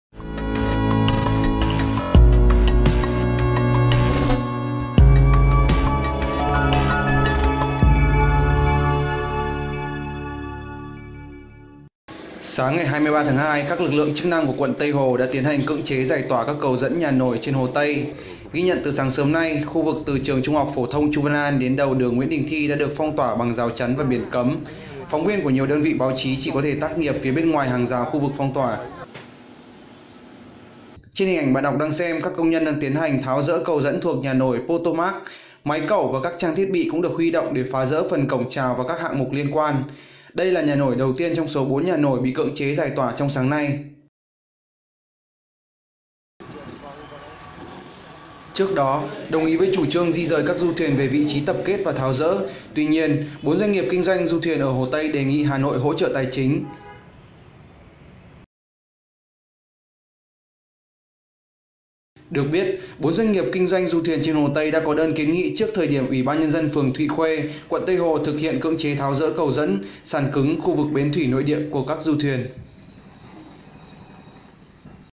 Bắt đầu cưỡng chế tháo dỡ cầu dẫn, sàn cứng nhà nổi Hồ Tây
Sáng ngày 23/2, các lực lượng chức năng của quận Tây Hồ đã tiến hành cưỡng chế giải tỏa các cầu dẫn nhà nổi trên Hồ Tây.
Ghi nhận từ sáng sớm nay, khu vực từ trường trung học phổ thông Chu Văn An đến đầu đường Nguyễn Đình Thi đã được phong tỏa bằng rào chắn và biển cấm. Phóng viên của nhiều đơn vị báo chí chỉ có thể tác nghiệp phía bên ngoài hàng rào khu vực phong tỏa.
Trên hình ảnh bạn đọc đang xem, các công nhân đang tiến hành tháo dỡ cầu dẫn thuộc nhà nổi Potomac, máy cẩu và các trang thiết bị cũng được huy động để phá dỡ phần cổng chào và các hạng mục liên quan. Đây là nhà nổi đầu tiên trong số 4 nhà nổi bị cưỡng chế giải tỏa trong sáng nay.
Trước đó, đồng ý với chủ trương di dời các du thuyền về vị trí tập kết và tháo dỡ, tuy nhiên, bốn doanh nghiệp kinh doanh du thuyền ở hồ Tây đề nghị Hà Nội hỗ trợ tài chính.
Được biết, 4 doanh nghiệp kinh doanh du thuyền trên hồ Tây đã có đơn kiến nghị trước thời điểm Ủy ban nhân dân phường Thụy Khuê, quận Tây Hồ thực hiện cưỡng chế tháo dỡ cầu dẫn, sàn cứng khu vực bến thủy nội địa của các du thuyền.